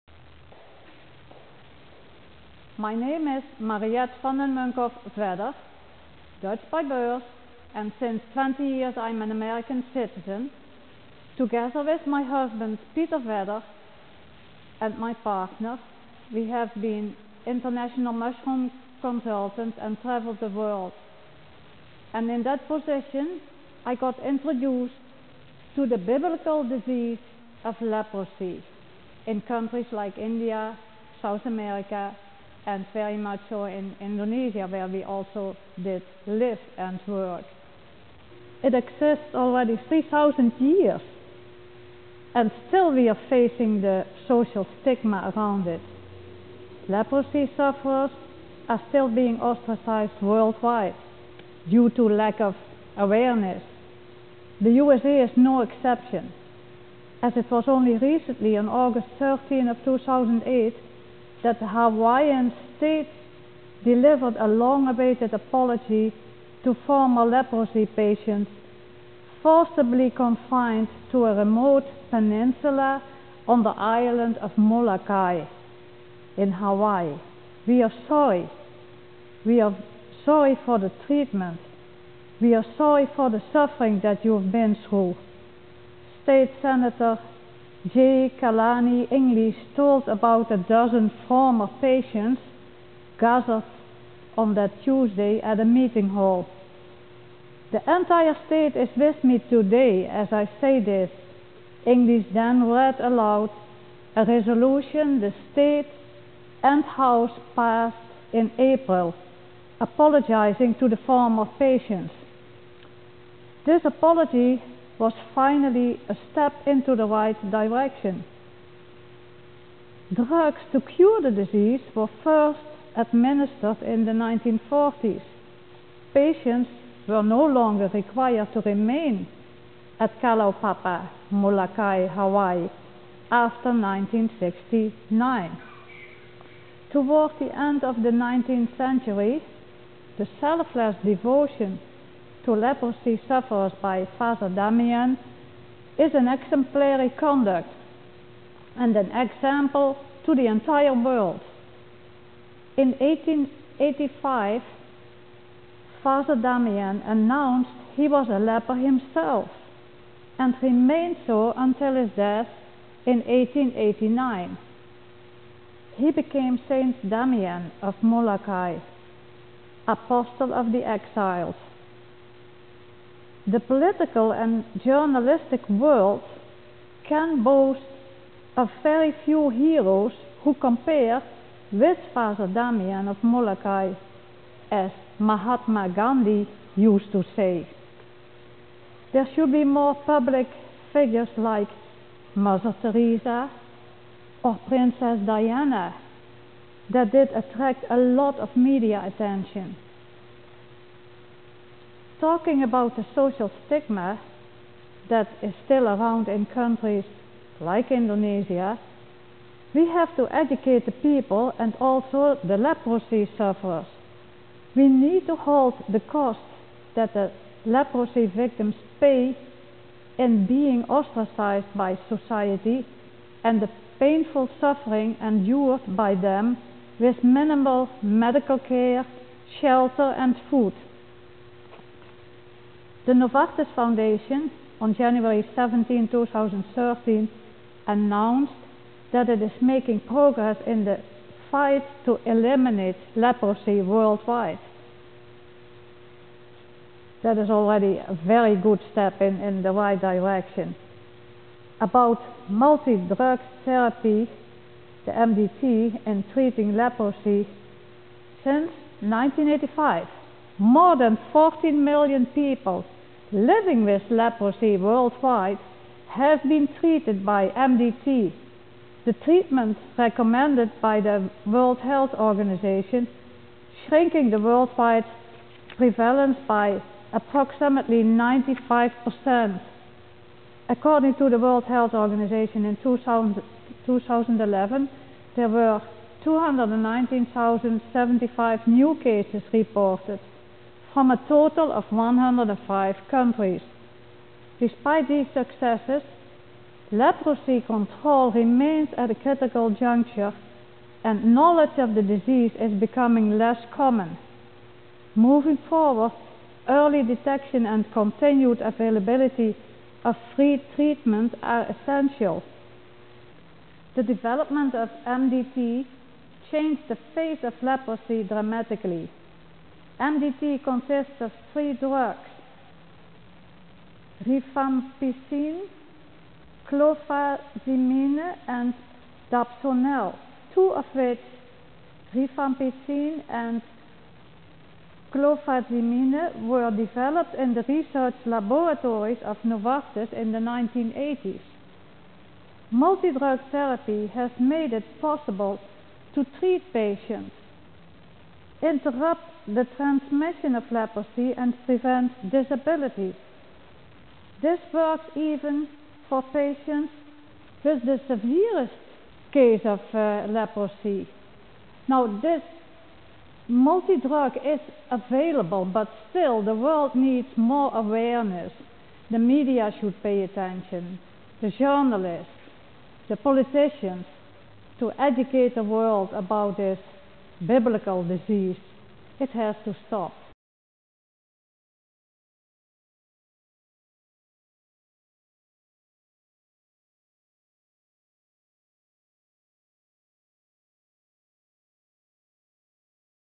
The Truth About Leprosy - Experiences by a USA Citizen for GPDLI & US EMBASSY Indonesia
Uploaded for the Meeting of 'The Truth About Leprosy', on Friday, February 28, 2014 in commemoration of World Leprosy Day.
Gerakan Peduli Disabilitas & Lepra Indonesia (Disability Care Movement & Leprosy Indonesia) together with @America from the US Embassy with thanks to Pak Nuah from GPDLI and Gil Gillis Video Studio from Dublin, Georgia/USA.